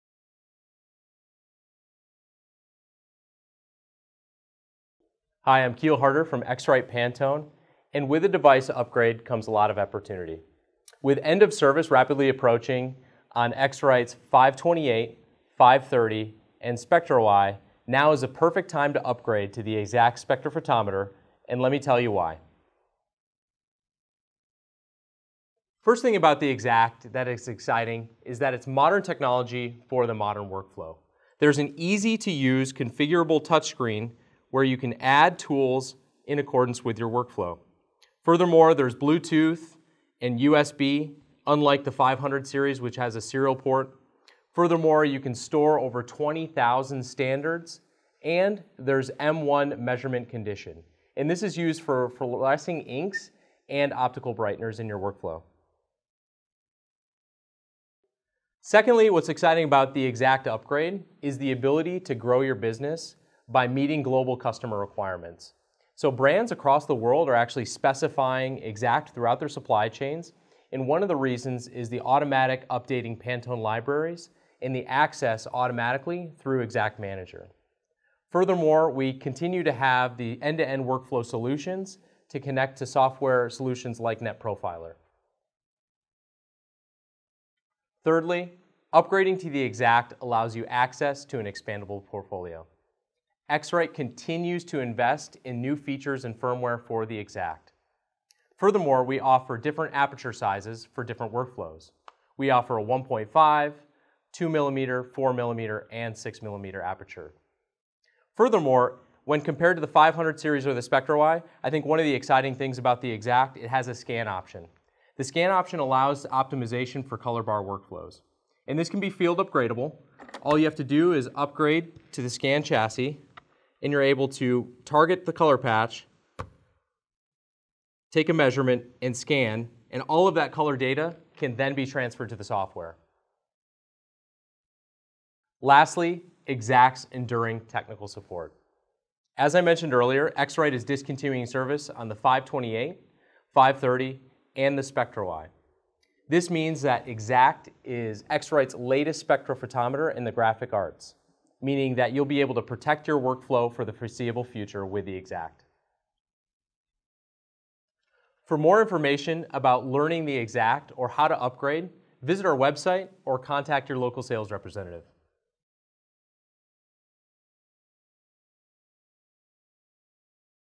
Top 4 Reasons to Upgrade to X-Rite eXact
With end of service approaching for the 528, 530, and SpectroEye, Product Manager Keal Harter demonstrates the top 4 reasons to upgrade to eXact.